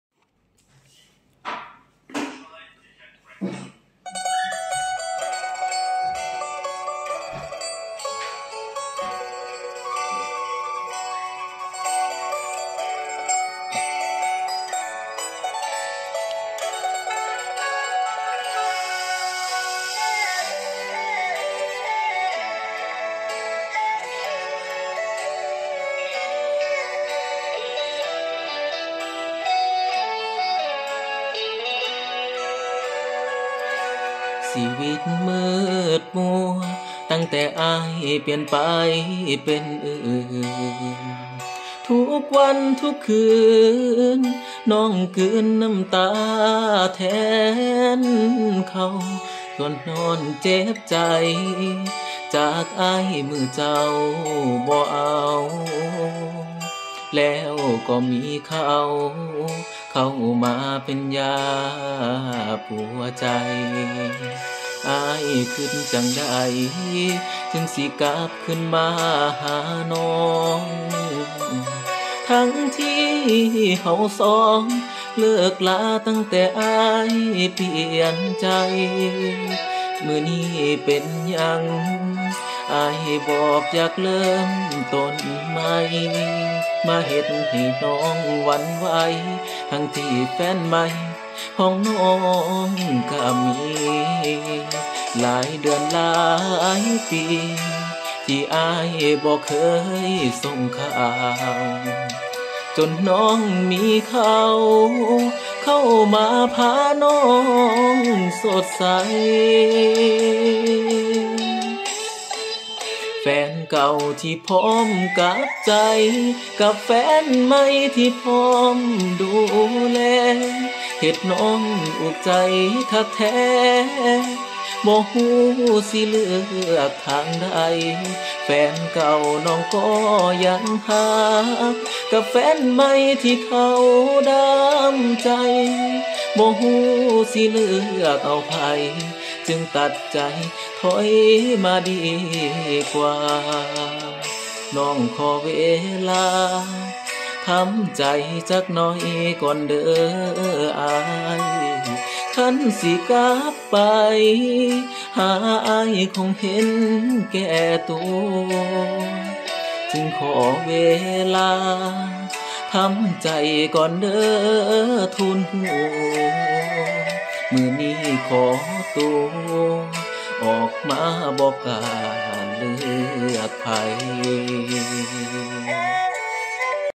ຫມາຍເລກ 07 ທ.ຫລີ່ງ ໂຄເວີເພງເລືອກ
ຫມາຍເລກ 07 ທ. ຫລີ່ງ ອາຍຸ 23 ປີ  , ບ້ານອີ່ຕູ້35 ເມືອງ ປາກຊ່ອງ ແຂວງ ຈຳປາສັກ